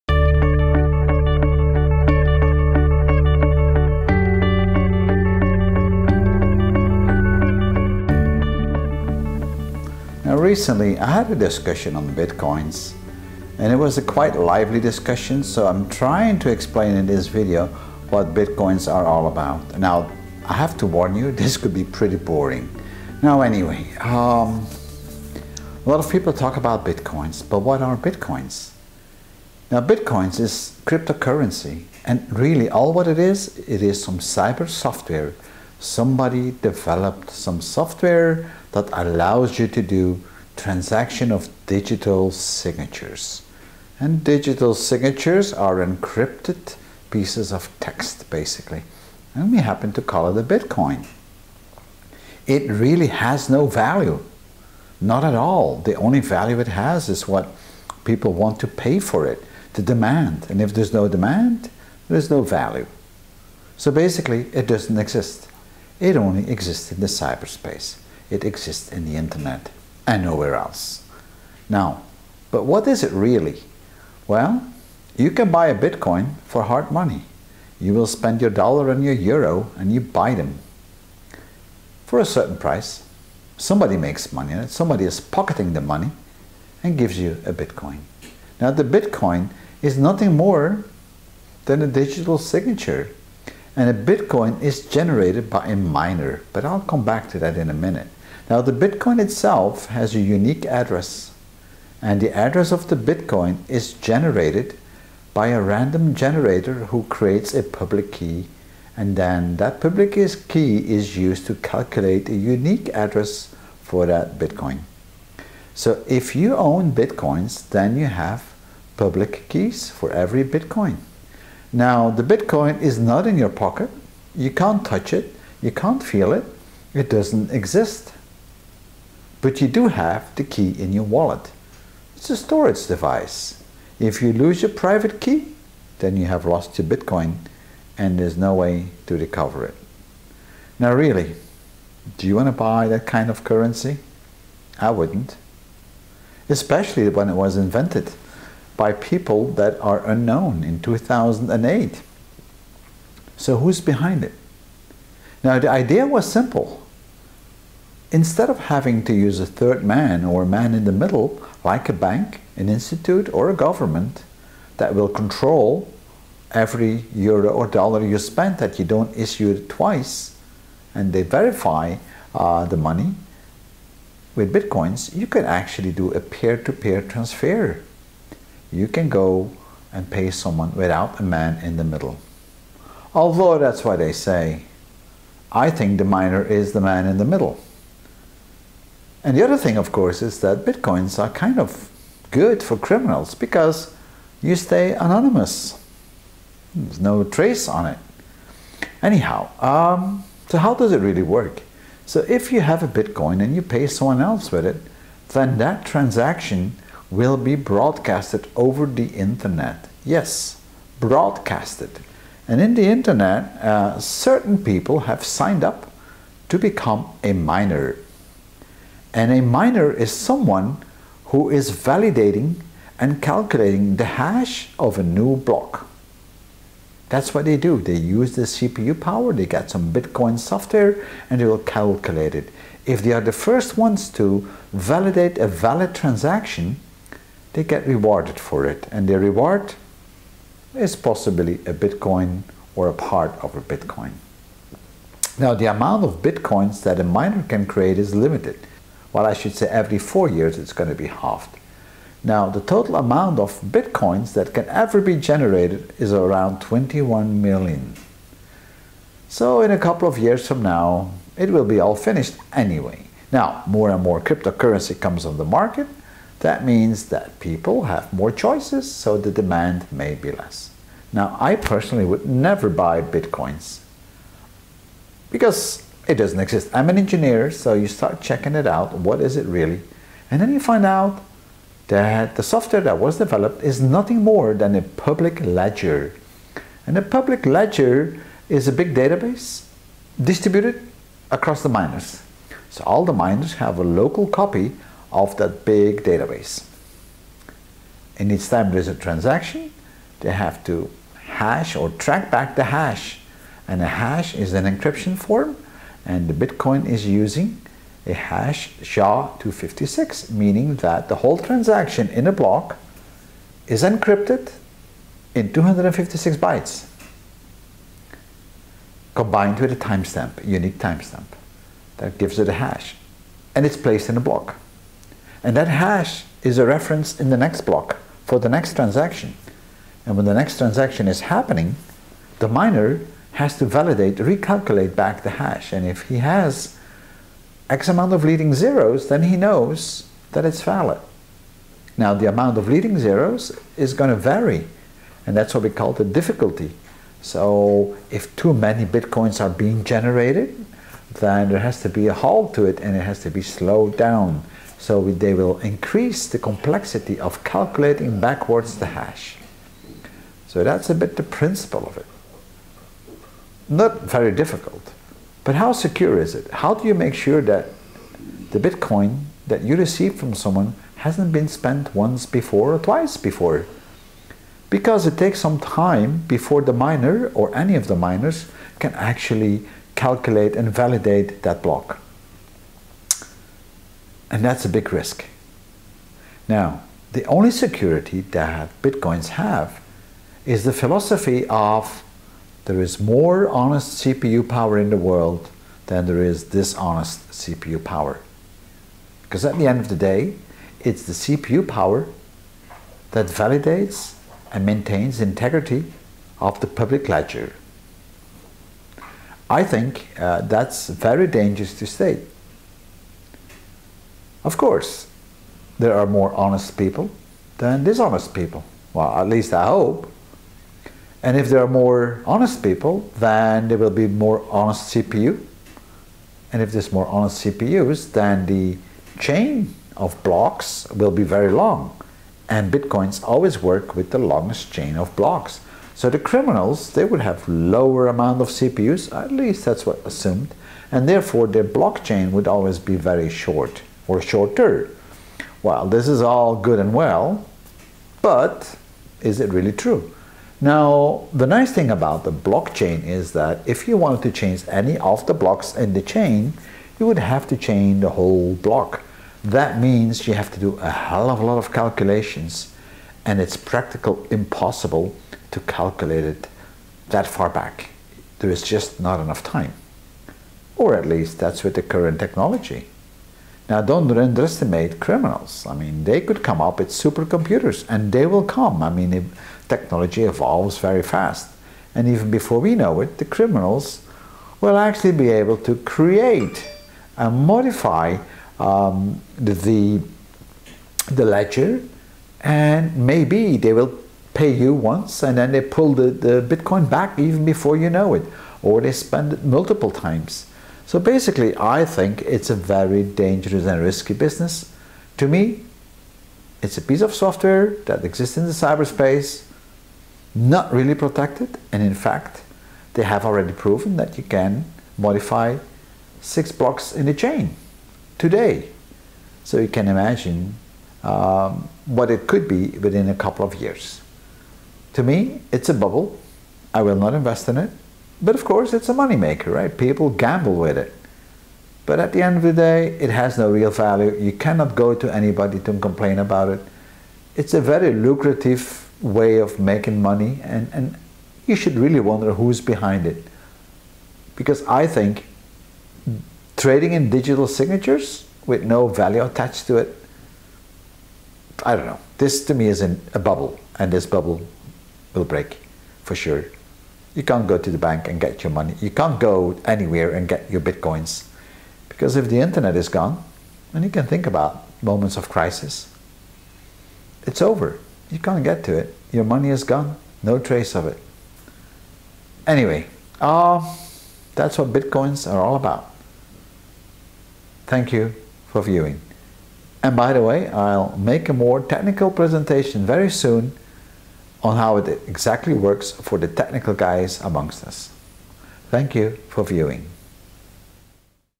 Bitcoin a software bubble based on digital signatures living in cyberspace
Whats wrong with the world ? Lest take the hype of crypto currency. here is a bit of what I did find out as an engineer on what it is, and guess what ? Its a bubble of digital signatures living in cyberspace